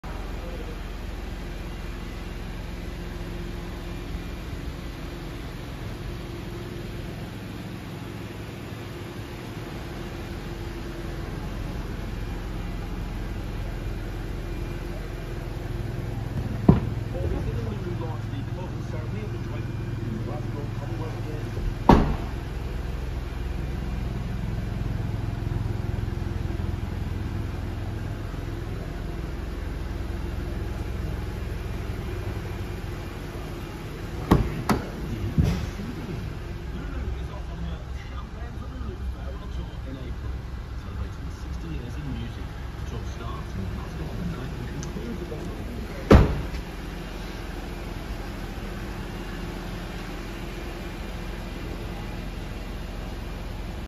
2024(73) Nissan Juke DIG-T ACENTA - Vic Young
Registration: NY73DPU
Mileage: 1,000 miles
Exterior Colour: RED
Engine Size: 1 L
Fuel Type: Petrol
Transmission: Manual
Bodystyle: Hatchback